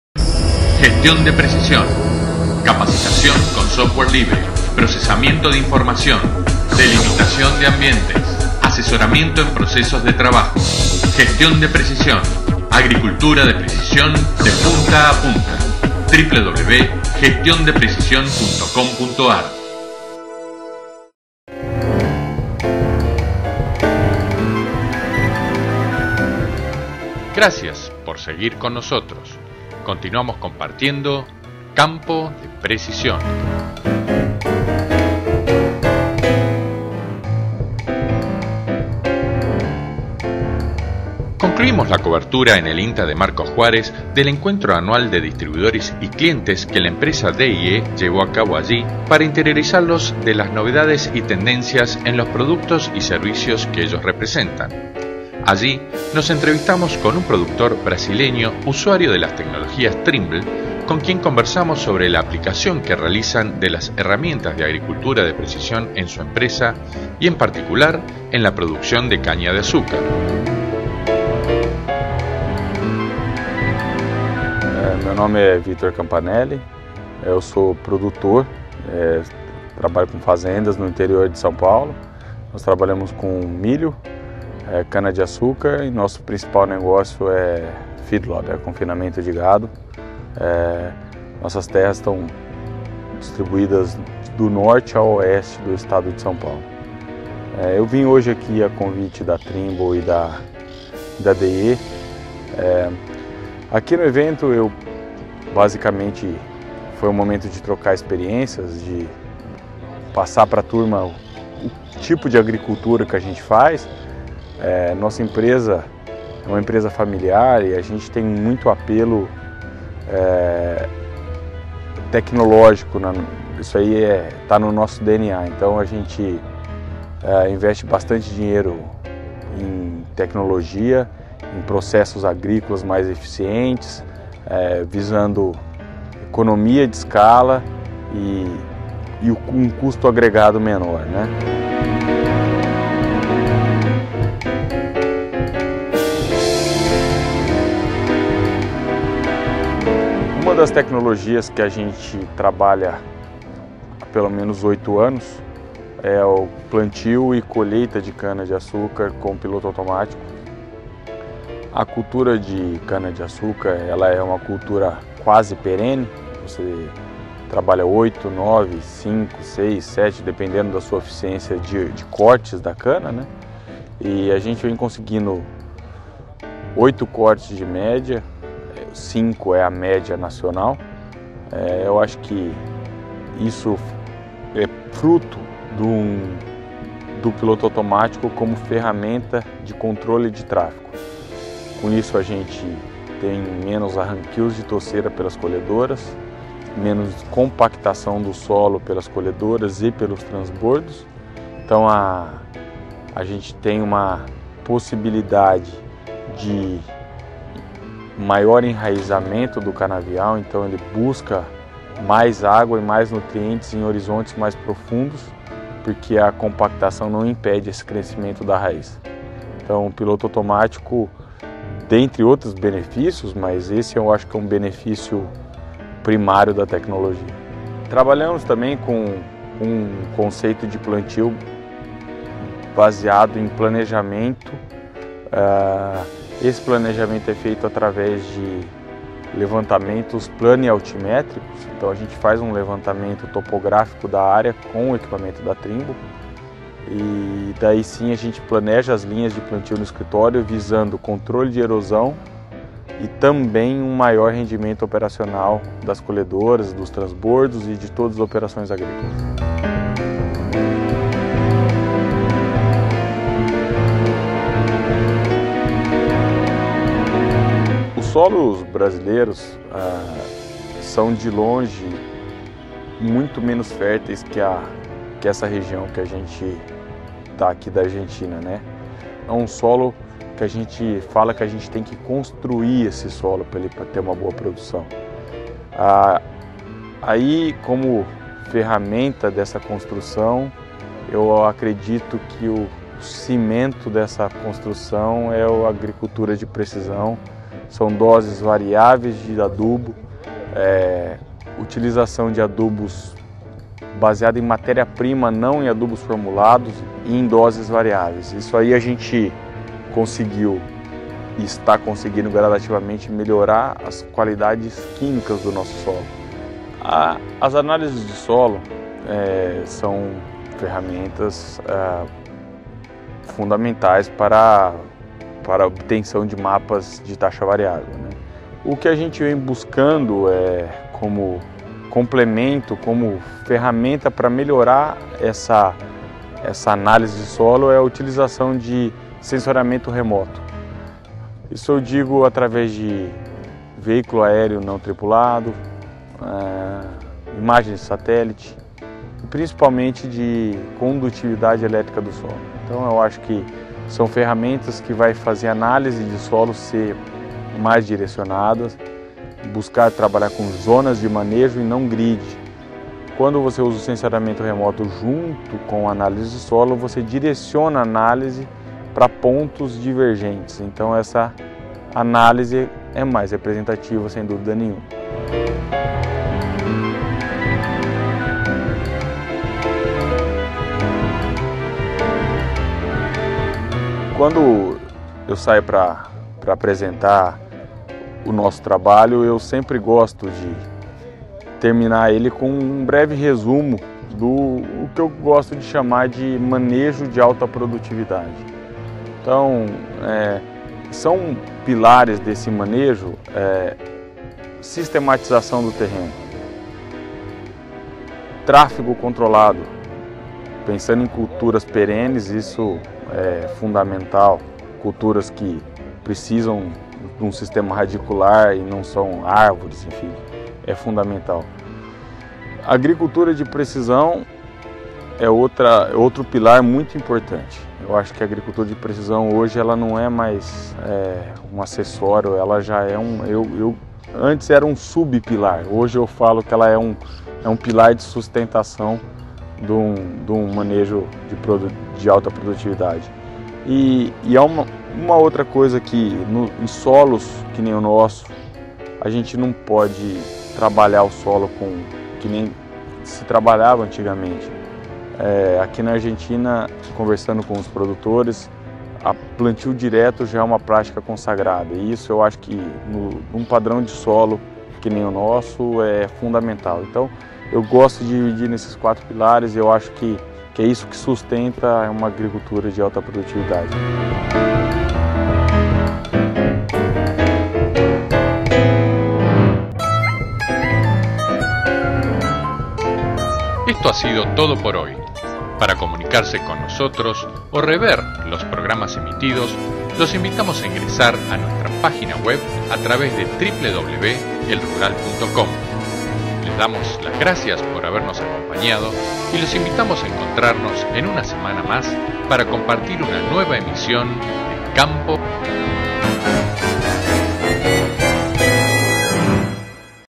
P328B3 Agricultura de precisión en caña de azucar
Campo de Precisión

Agricultura de precisión en caña de azucar

Víctor Campanelli
Productor -- Paschoal Campanelli

Concluímos la cobertura en el INTA de Marcos Juarez, del encuentro anual de distribuidores y clientes que la empresa D&E llevó a cabo allí, para interiorizarlos de las novedades y tendencias en los productos y servicios que ellos representan.
Allí, nos entrevistamos con un productor brasileño usuario de las tecnologías Trimble, con quien conversamos sobre la aplicación que realizan de las herramientas de agricultura de precisión en su empresa y en particular, en la producción de caña de azúcar.